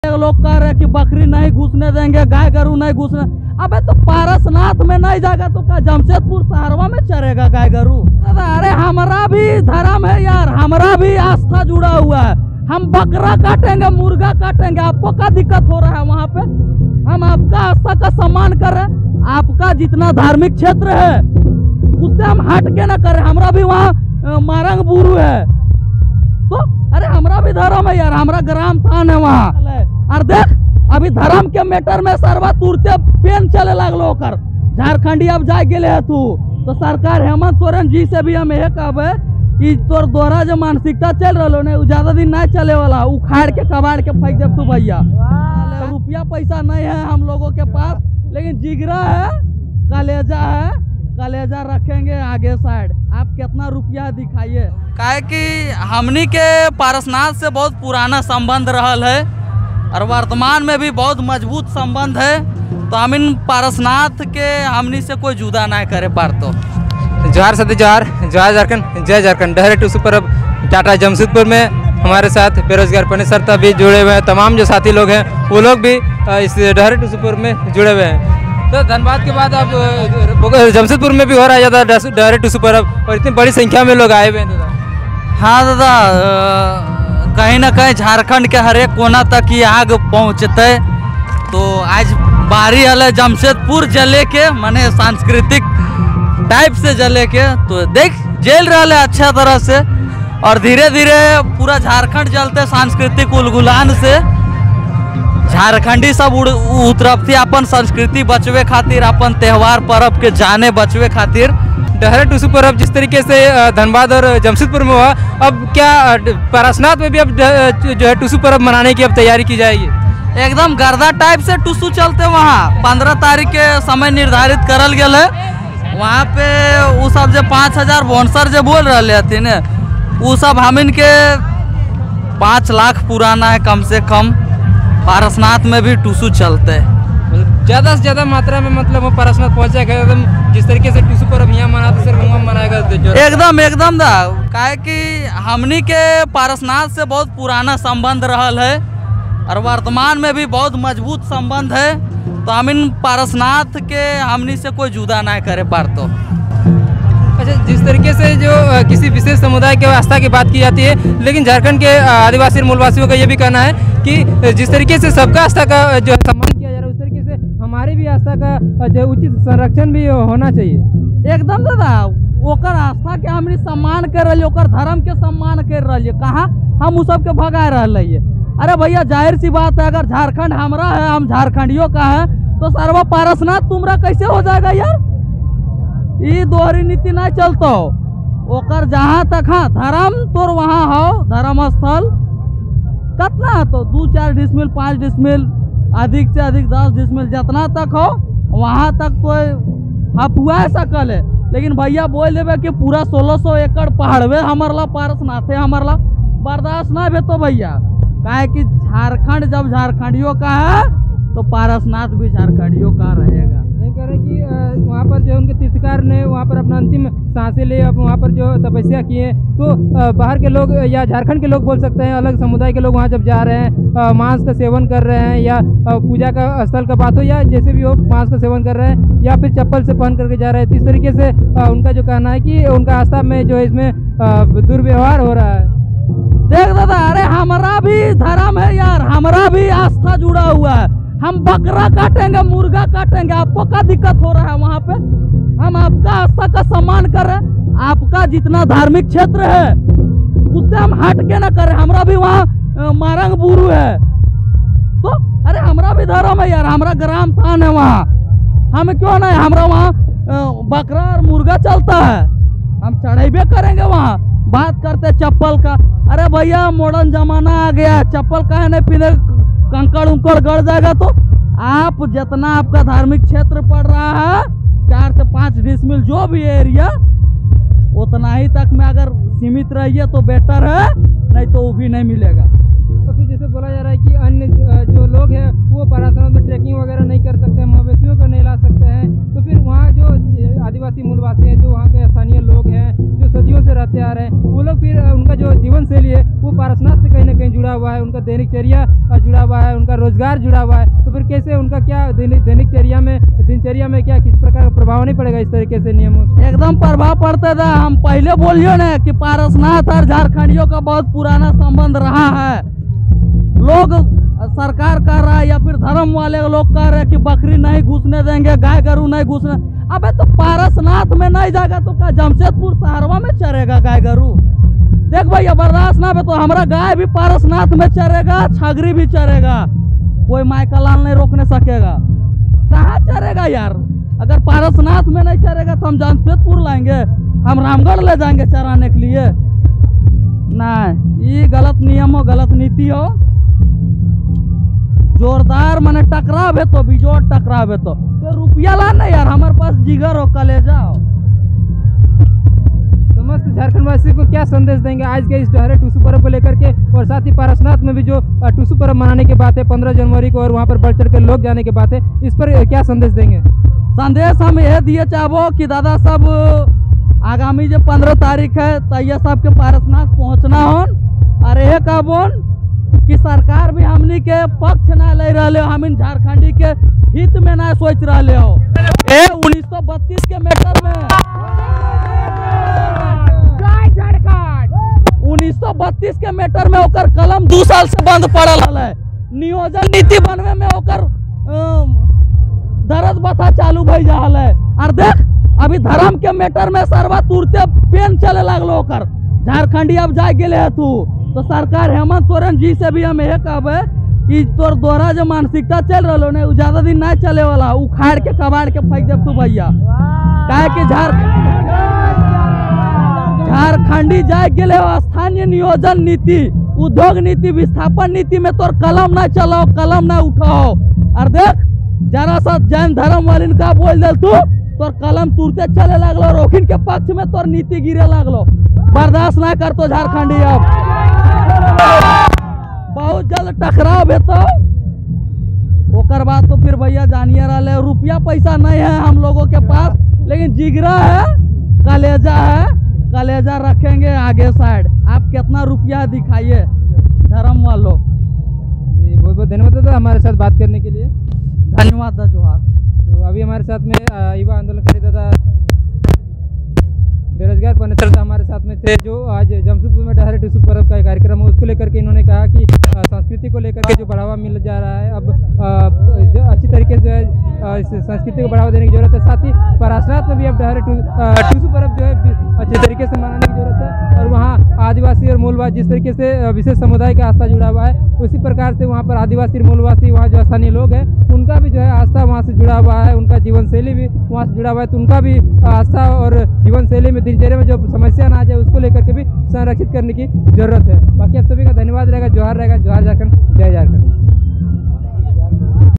पारसनाथ वि'वाद पर जोरदार इंटरव्यू बेरोजगार पनेश्वर Parasnath Vivad Jharkhand News Dahare Tusu Parab
जोहार 🙏 दोस्तों आप सभी का हमारे YouTube Channel में स्वागत है।

पारसनाथ वि'वाद पर जोरदार इंटरव्यू बेरोजगार पनेश्वर |  Parasnath Vivad Jharkhand News Dahare Tusu Parab Jamshedpur News

वीडियो को ज्यादा से ज्यादा शेयर कीजिए 🙏🙏

Produced By ► Jitendra Mahato

Google ► Jitendra Vlogs